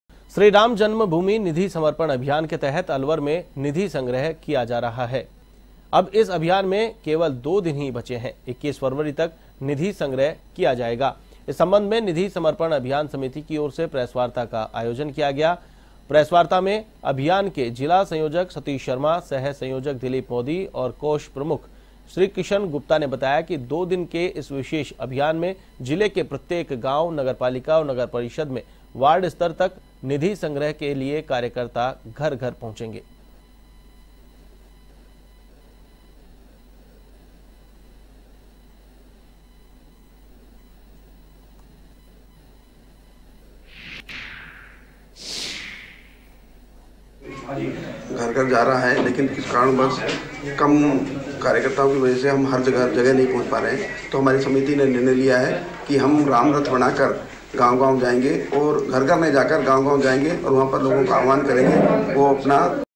राम मंदिर निधि समर्पण अभियान 21 फरवरी तक, कार्यकर्ता जाएंगे घर घर
A1TV  | Rajasthan News | Hindi News